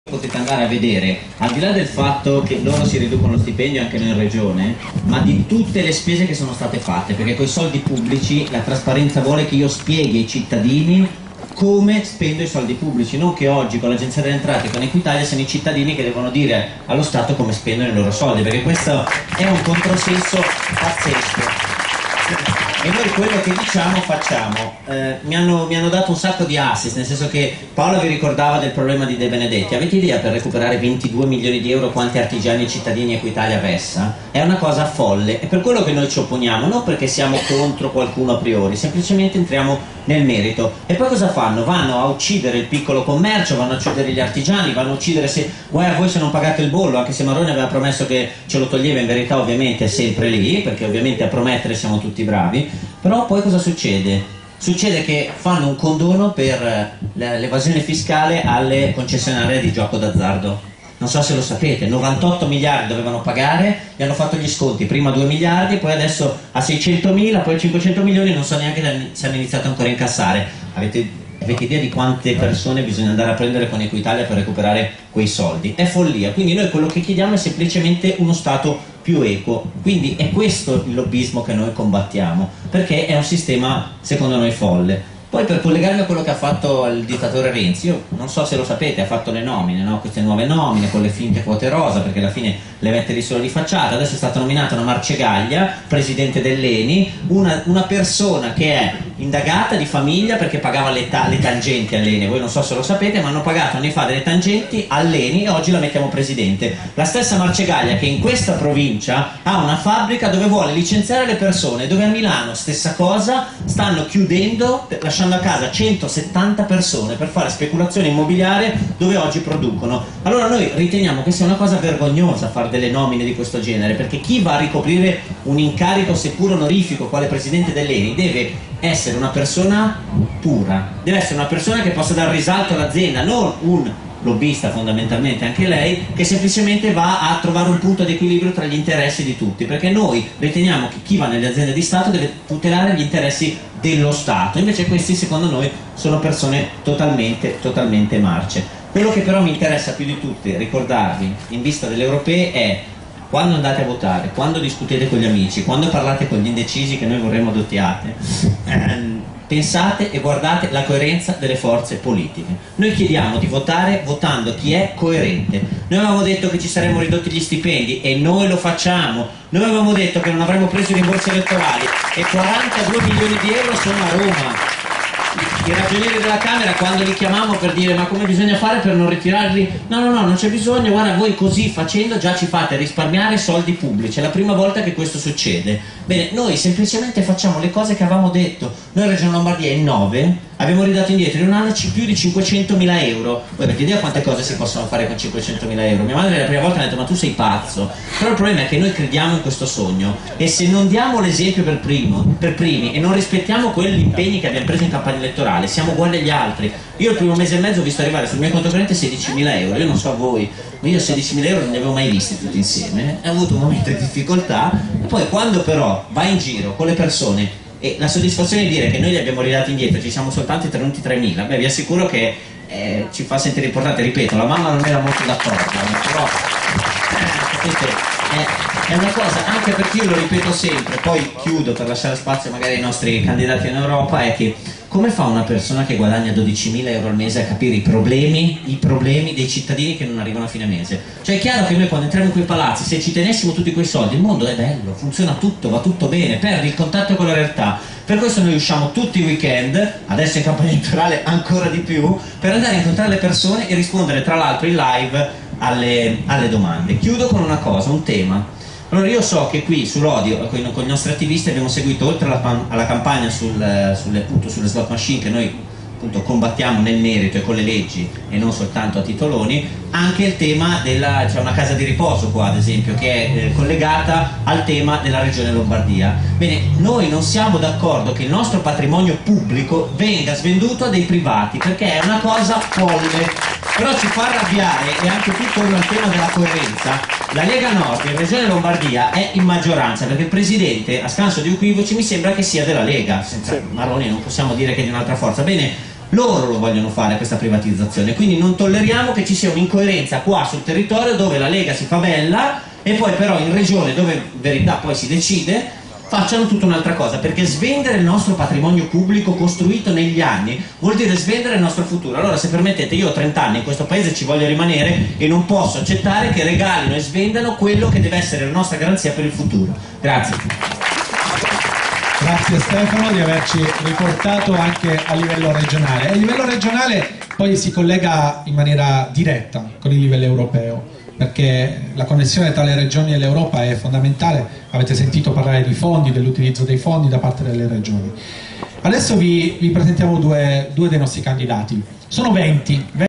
VinciamoNoi Tour - Stefano Buffagni - Lodi
Intervento del Consigliere Stefano Buffagni alla tappa di Lodi del  Vinciamo Noi Tour (19 aprile).